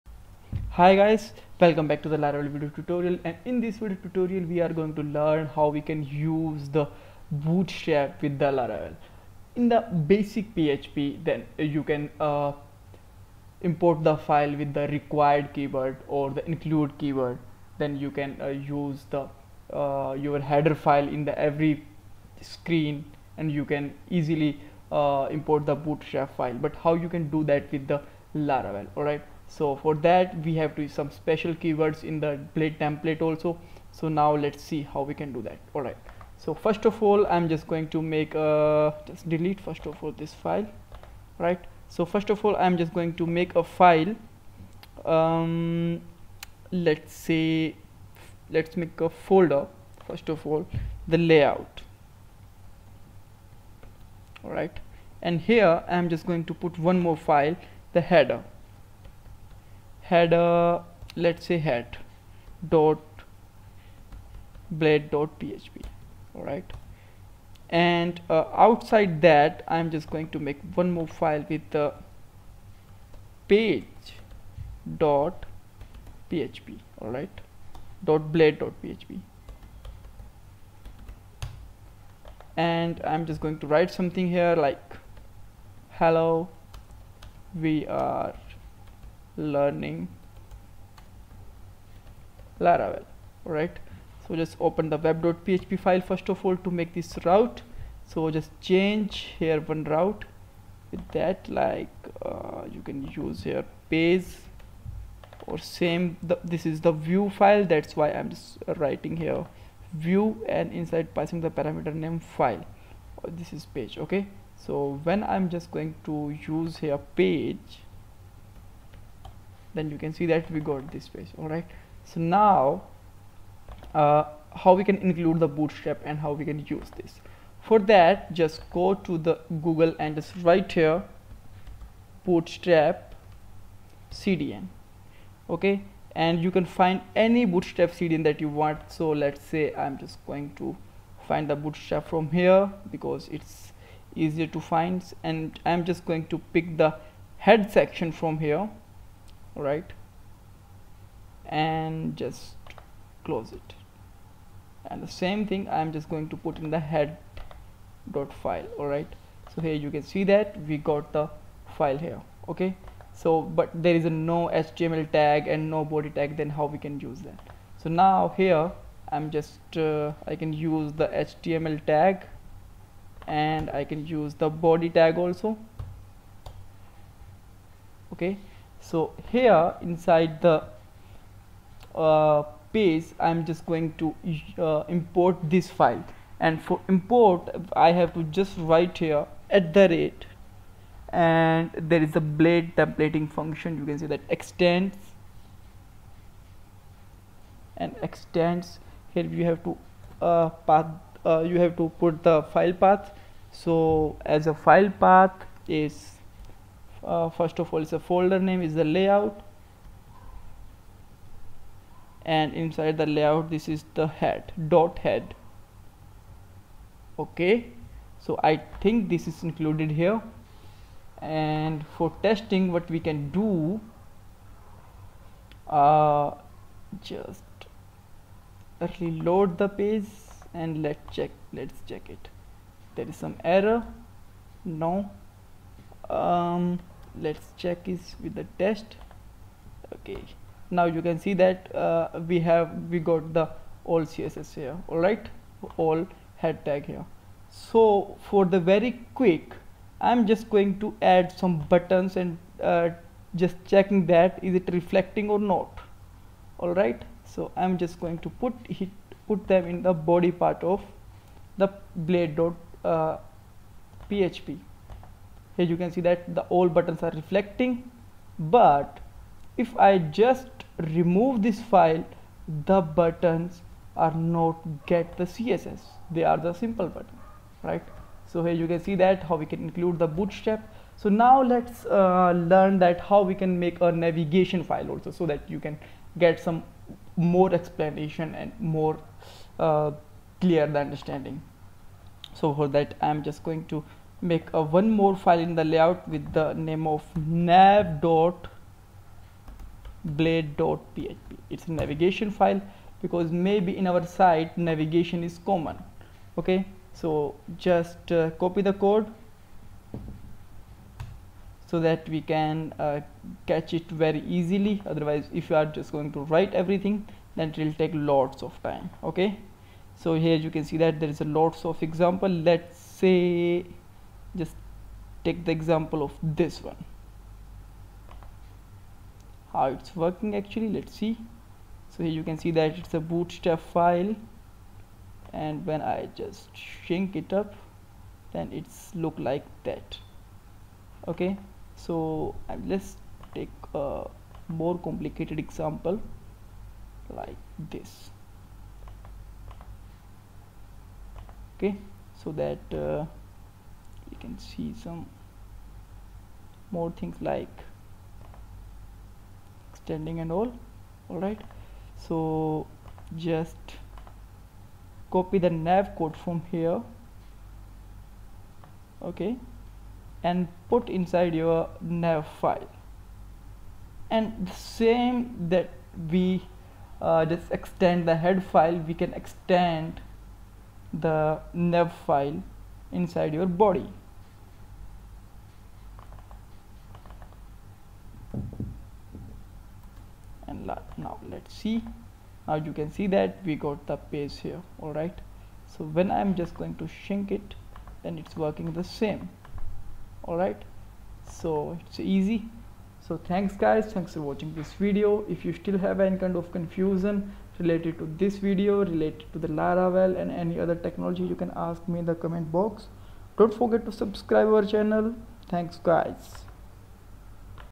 Laravel 5.8 tutorial - use Bootstrap
In this laravel 5.8 video tutorial we will learn how to use Bootstrap with laravel